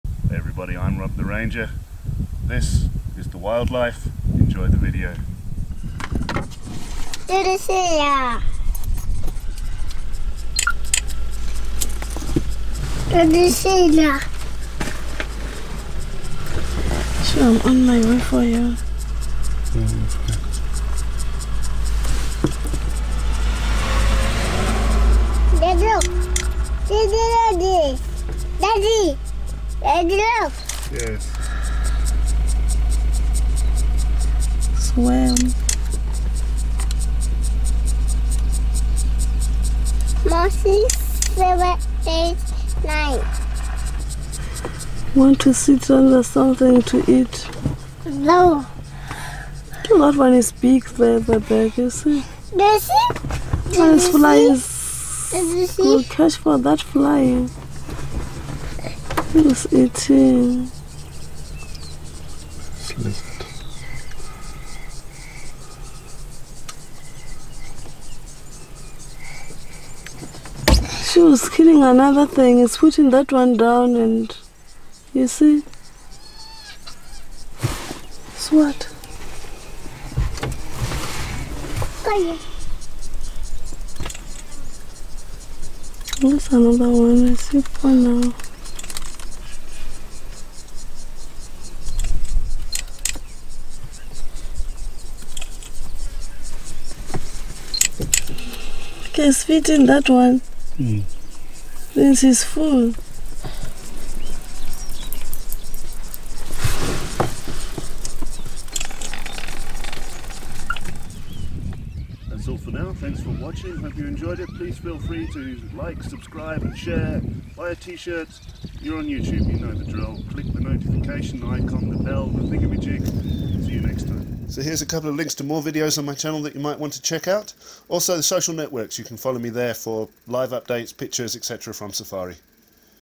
Southern Ground Hornbills | Kruger National Park
A nice sighting of some endangered Southern Ground Hornbills foraging for food in the Kruger.
Here are some facts about these lovely birds:
Ground hornbills call together before dawn in a chorus of repeated low grunting notes that sounds not unlike a distant lion. They amplify their calls by inflating the big, red, balloon-like wattle below their bill.
Small animals need to lie low when a party of ground hornbills is out foraging because these omnivores snap up anything – from insects and lizards to small birds, rodents, tortoises and snakes as big as puff adders.
Ground hornbills are very slow breeders and, as a result, a pair produces just one brood of two chicks every nine years, only one of which survives. Immature birds within the social group work as ‘helpers’, caring for the single chick.
Ground hornbills have lived up to 70 years in captivity. This makes them one of the world’s longest-lived birds, on par with albatrosses.
Since traditional African cultures saw ground hornbills as harbingers of rain, killing them was taboo. Thus, sadly, with the passing of such beliefs, these birds have become increasingly threatened.

Filmed in the Kruger National Park, South Africa